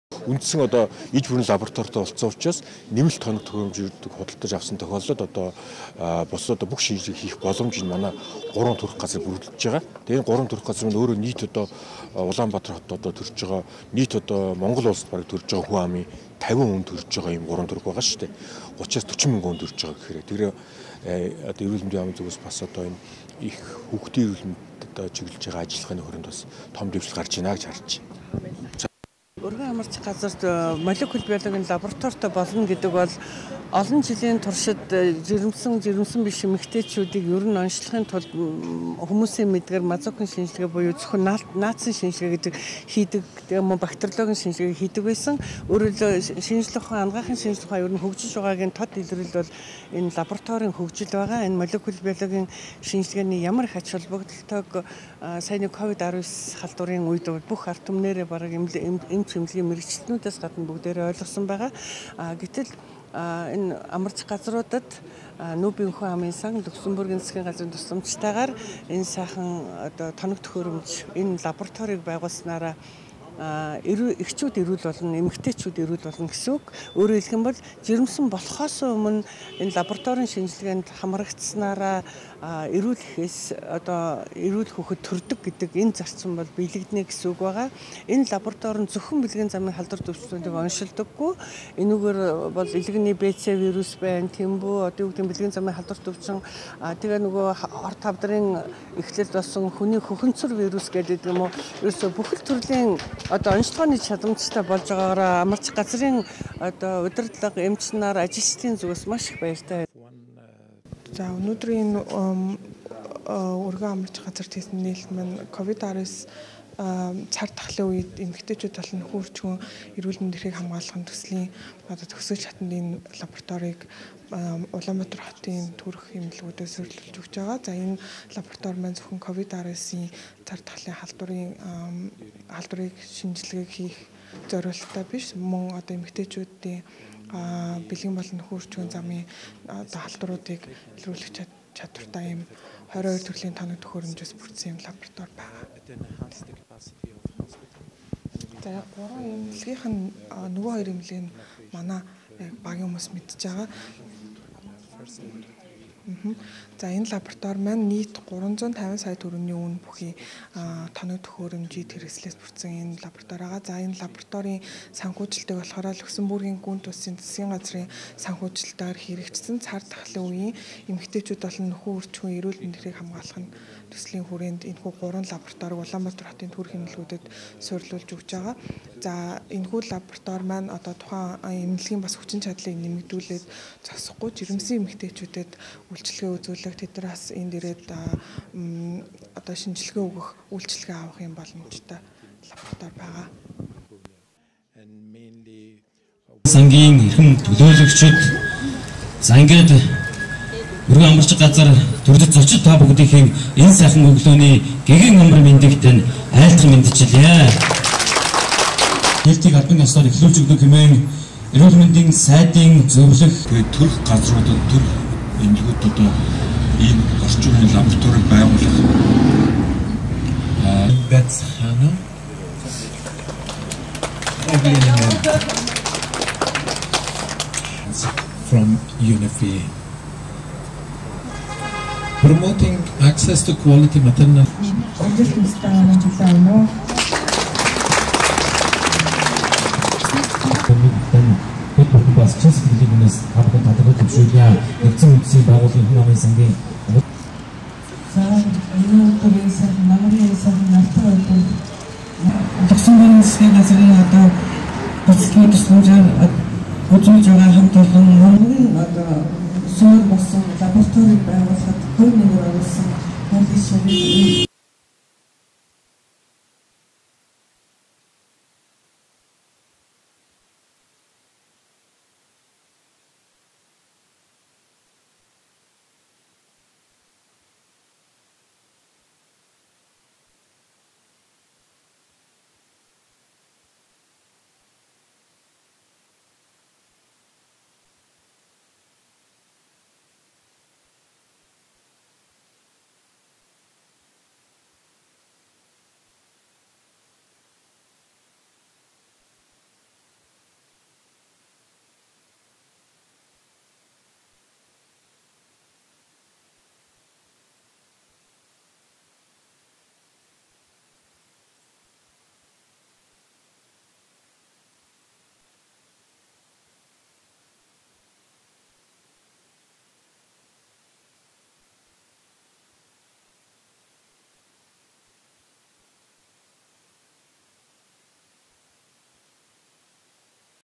09 19 Urguu amarjih lobrator
О.Батбаяр Эрүүл мэндийн сайдын зөвлөх
Ж.Энхцэцэг Өргөө амаржих газрын дарга
Халид Шарифи НҮБ-ын Хүн амын сангийн Монгол дахь төлөөлөгчийн газрын дарга, доктор